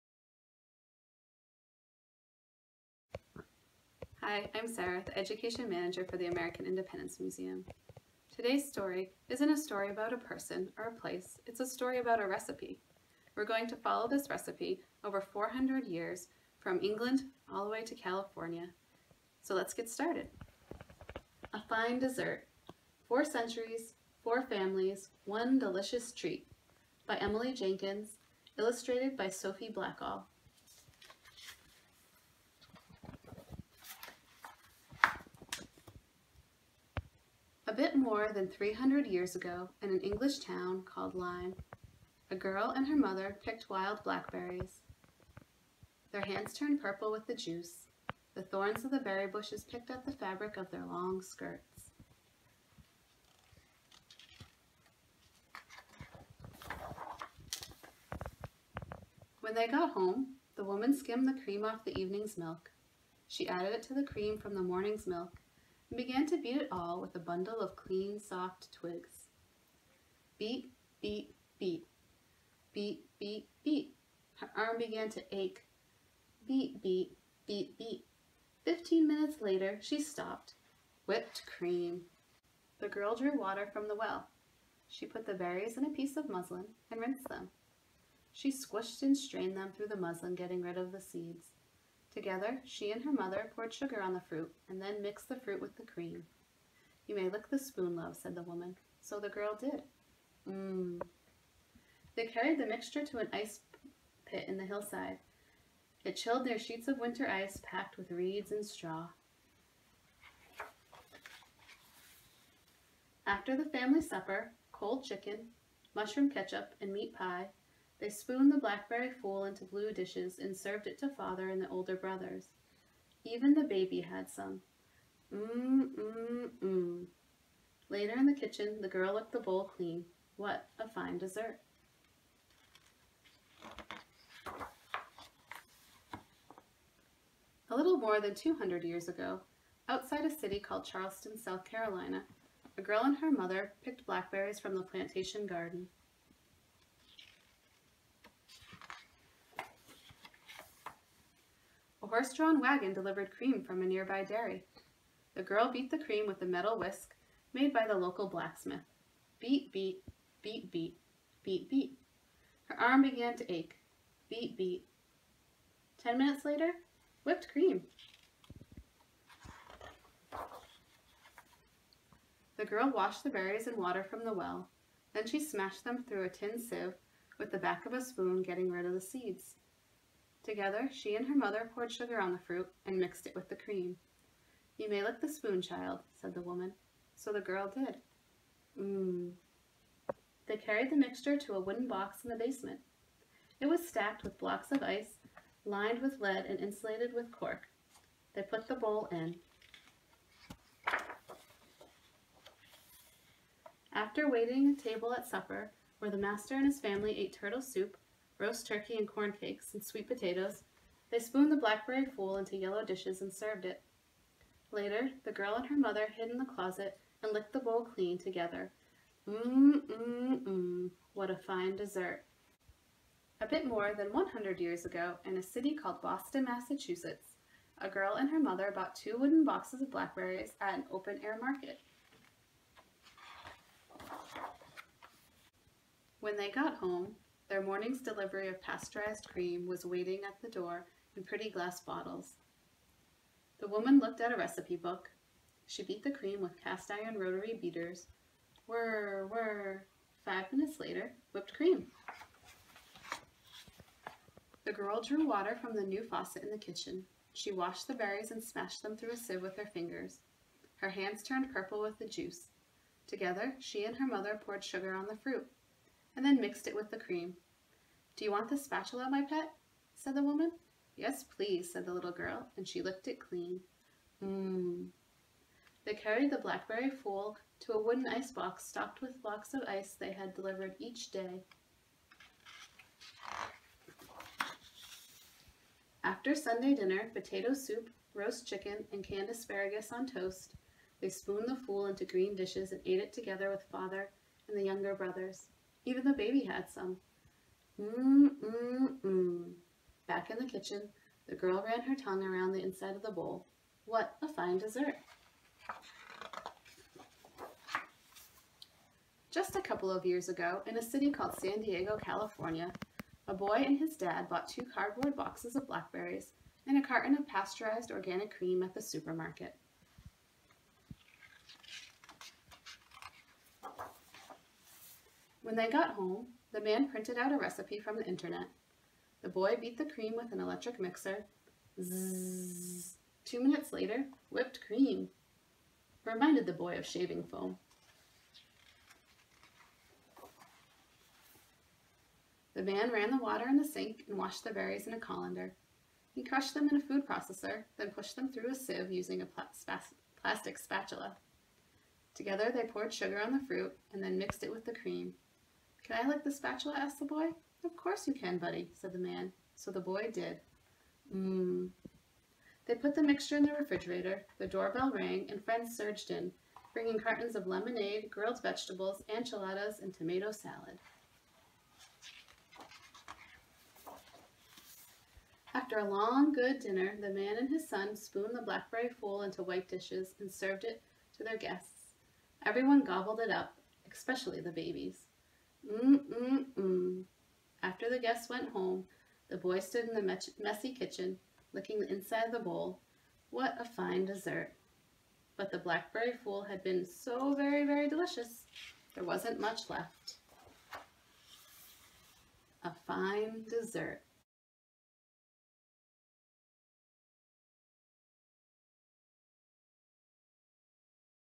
Revolutionary Story Time - A Fine Dessert
Our education manager shares the book "A Fine Dessert: Four Centuries, Four Families, One Delicious Treat" by Emily Jenkins and illustrated by Sophie Blackall.  Published by Schwartz & Wade (January 27, 2015).